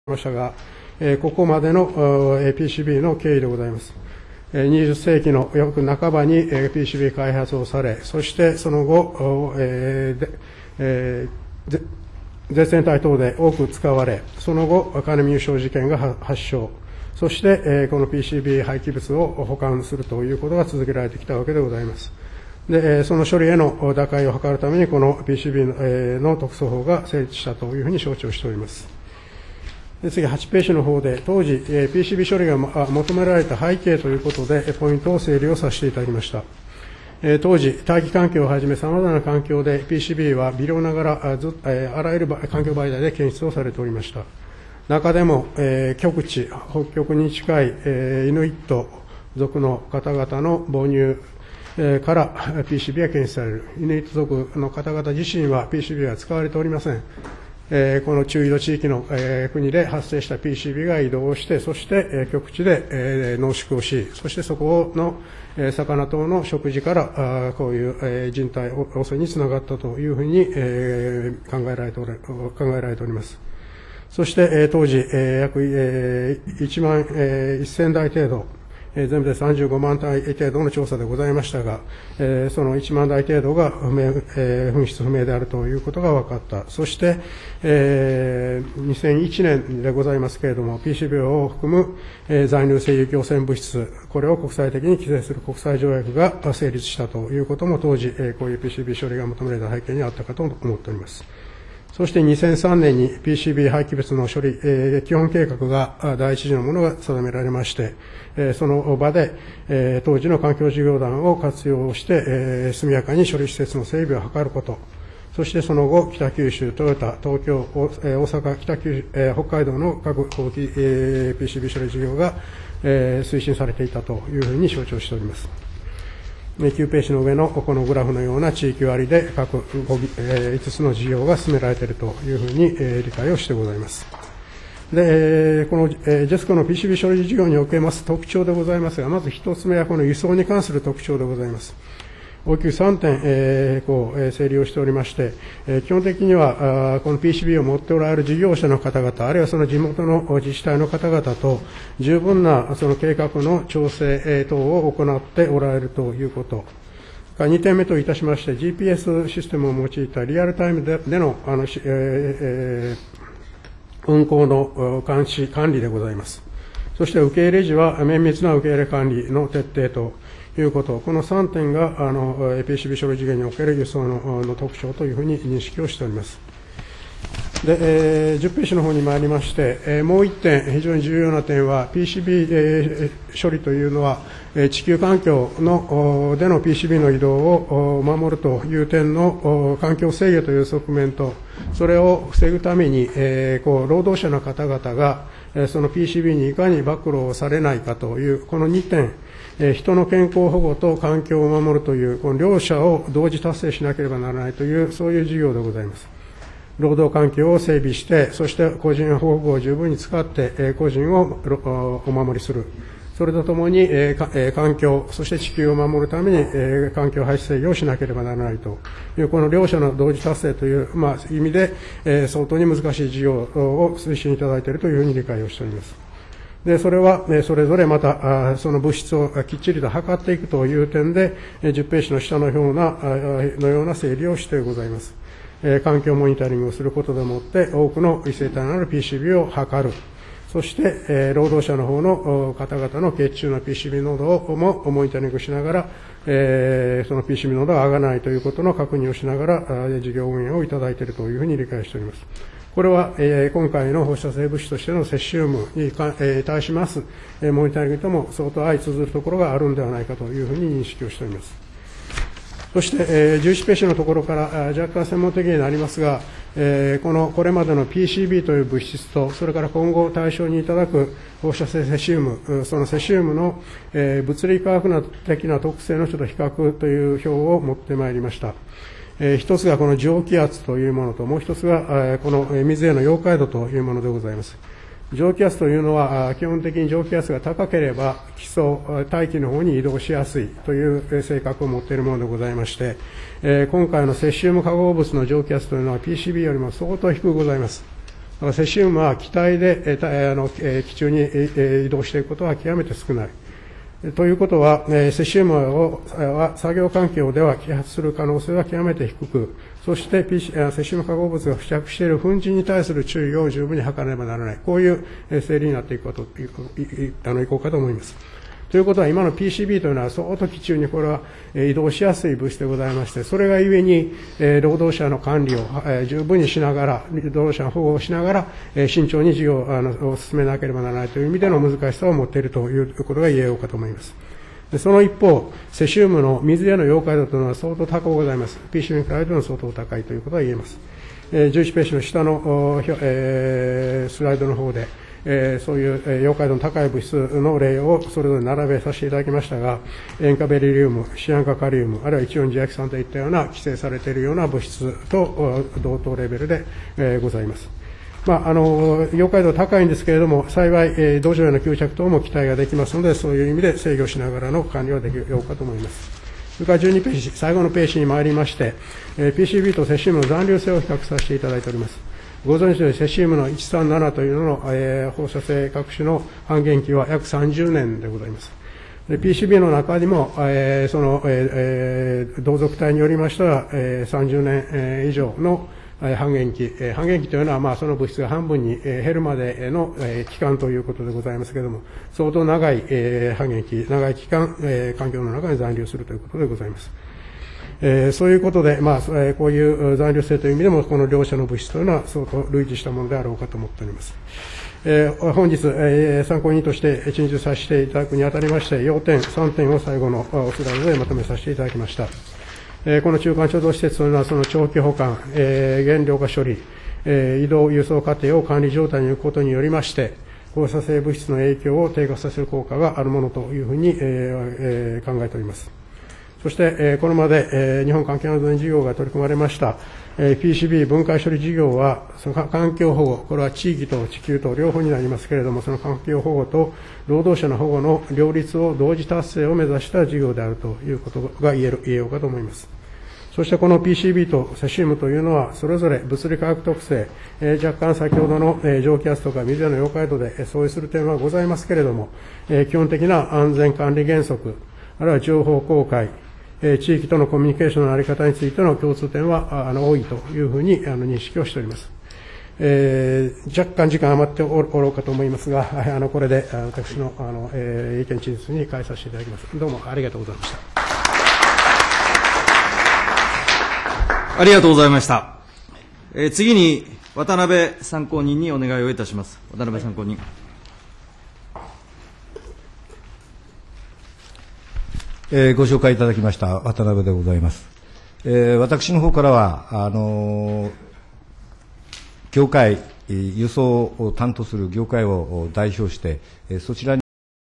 PCBとセシウム 衆院・環境委員会10/28
10/28 衆院・環境委員会　ポリ塩化ビフェニールＰＣＢとセシウム　参考人/京大・酒井伸一(京都大学環境科学センター・センター長)